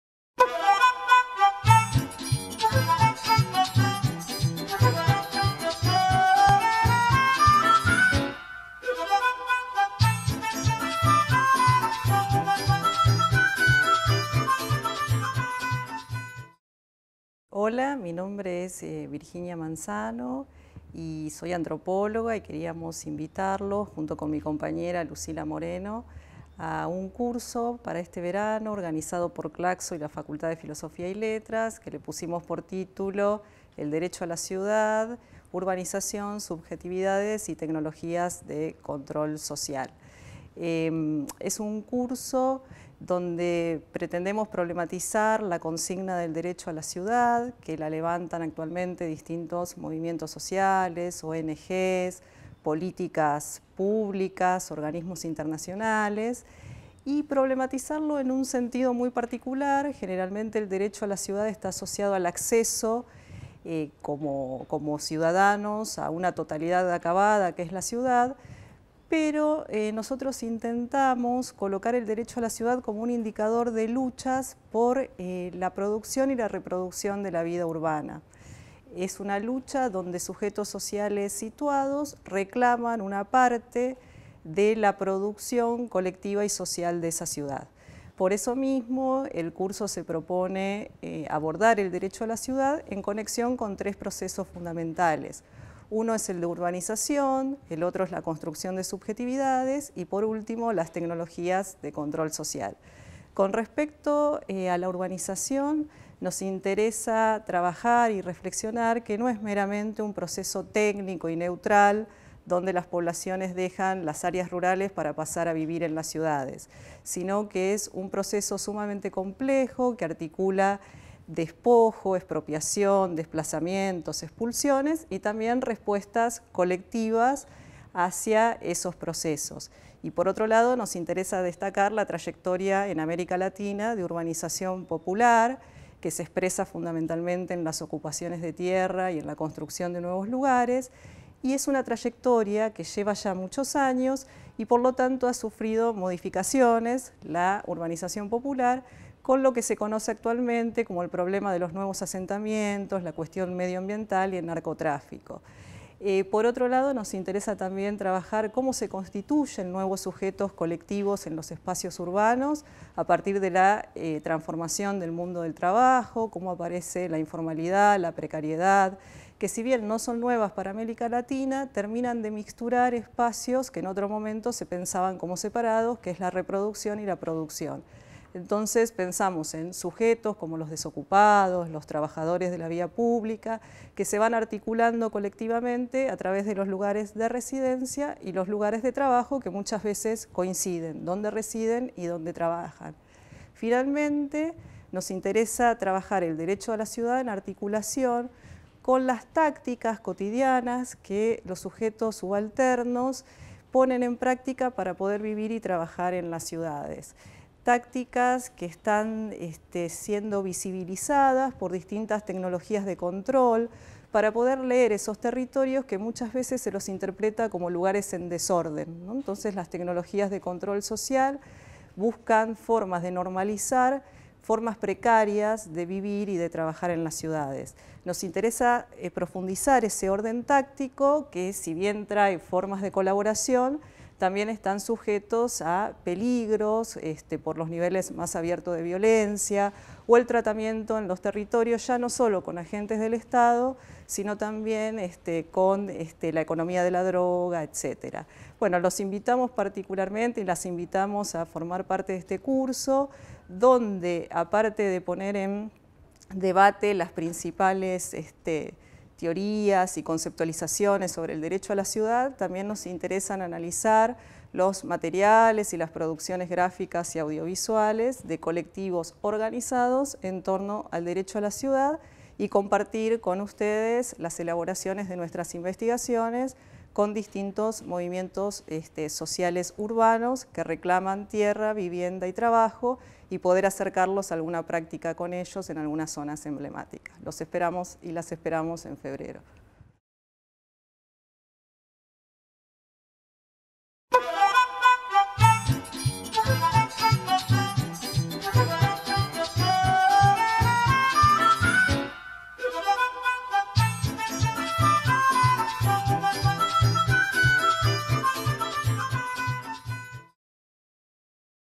Seminario 9 - El derecho la ciudad: urbanización, subjetividades y tecnologías de control
Profesoras:

Virginia Manzano. Doctora de la Universidad de Buenos Aires, área antropología social. Profesora en la Universidad de Buenos Aires. Dictó cursos de posgrado en la Universidad Nacional de Rosario, en la Universidad Nacional de La Plata, en la Universidad Nacional de Salta, en la Universidad Nacional de General Sarmiento y en la Universidad del Salvador.

Lucila Moreno. Licenciada en Ciencias Antropológicas y Profesora por la Facultad de Filosofía y Letras de la Universidad de Buenos Aires.

Profesora invitada: Dra. María Cristina Cravino
Institución a la que se realizará una visita: Red de Hábitat de Quilmes “Juanjo Cantiello” (San Francisco Solano).

Horario de cursada: 17 a 21.30 hs.